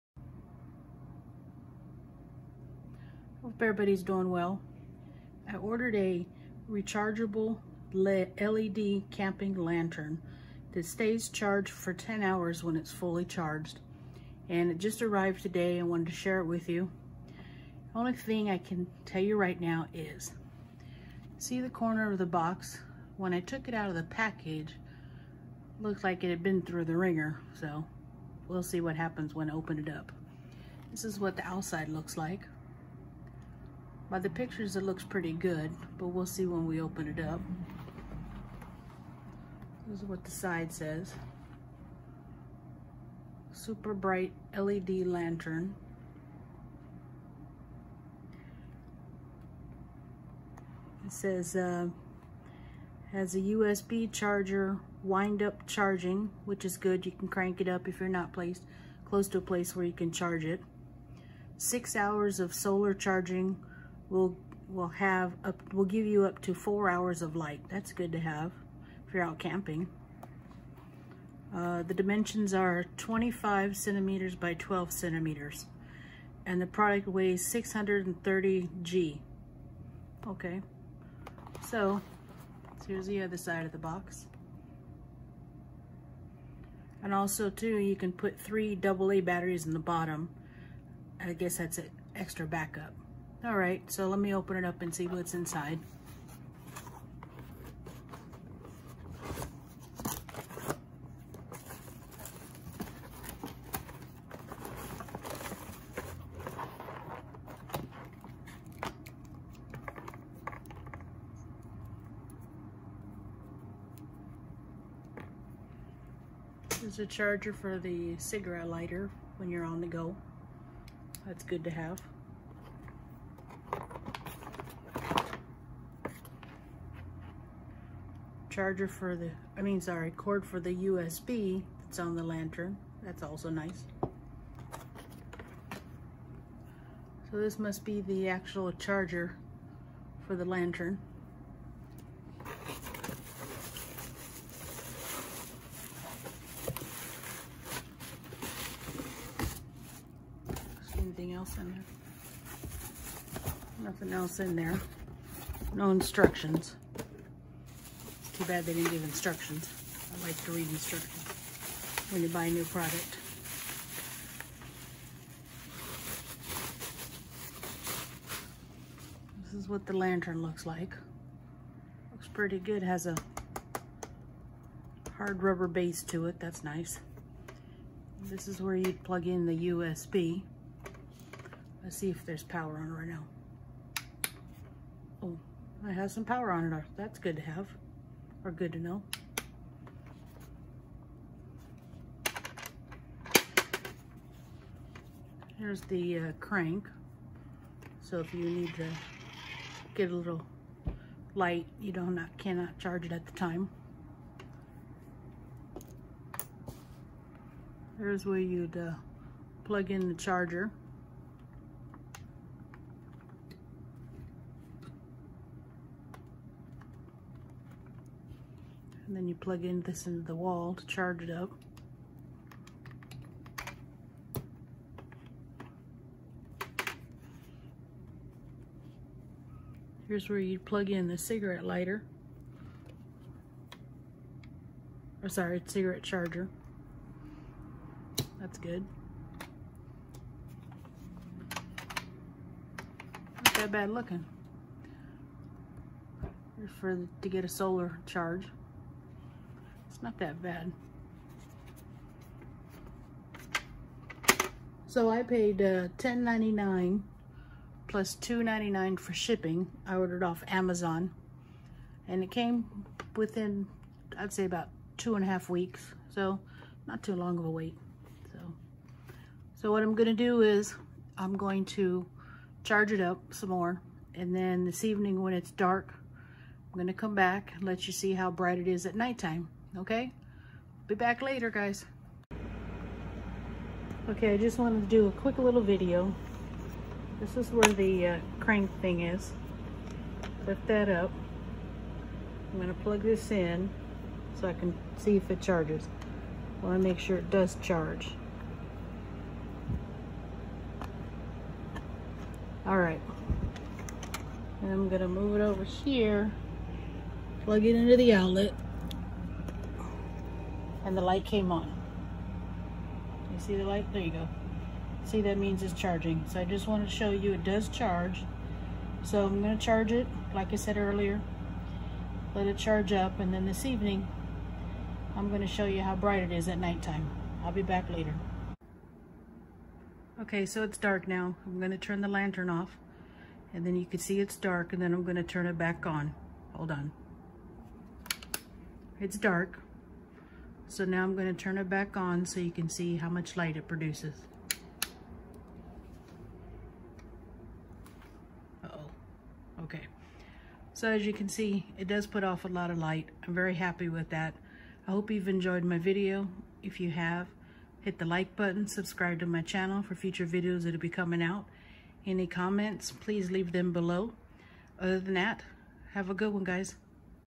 Jadpes Rechargeable Lantern Solar Powered. Unboxing & Review
Jadpes rechargeable lantern. Can be charged by solar (8hours) or hand crank or car adapter or AC adapter  or USC cable. Also you can put 3 AAA batteries in the bottom. When fully charged it has 10 hours of light. I ordered it on Amazon for $10.38 plus $2.99 for shipping. Of course plus tax.